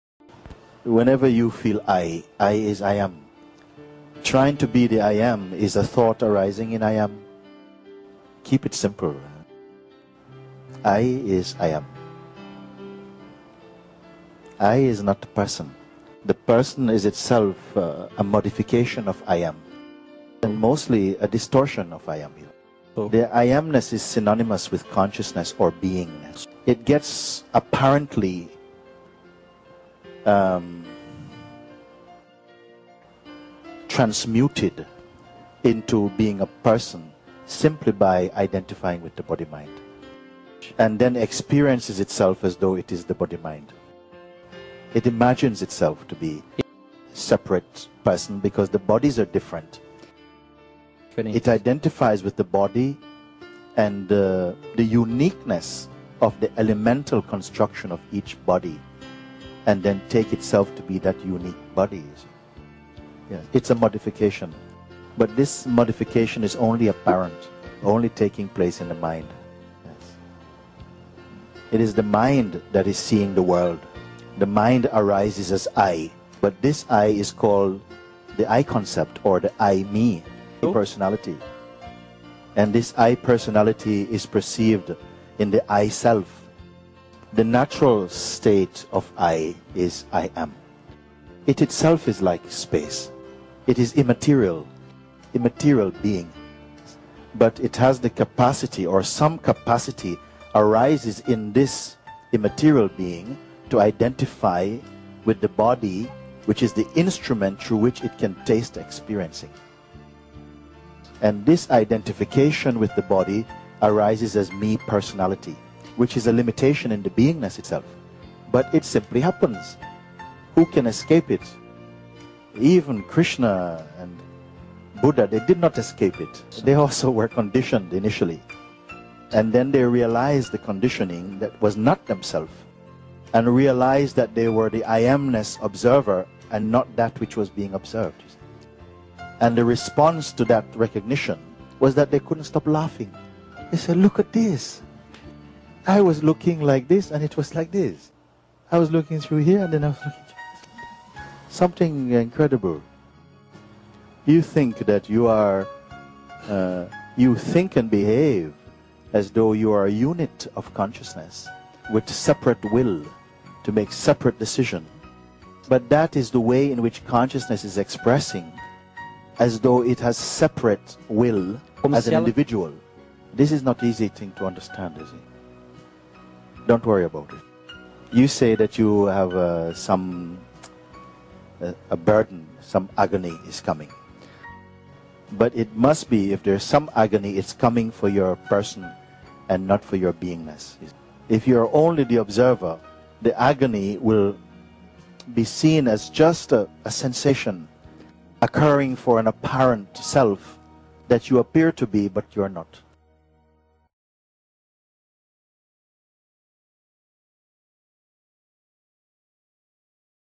LIFE AWAKENING~WORLD AS " I-PERSONALITY"
WORLD AS " I-PERSONALITY"~~~LIFE AWAKENING~
Wise Words by Mooji JI. This video is a tribute to Mooji Ji
THANK YOU Mooji JI: it is a privilege for all of us to share this message.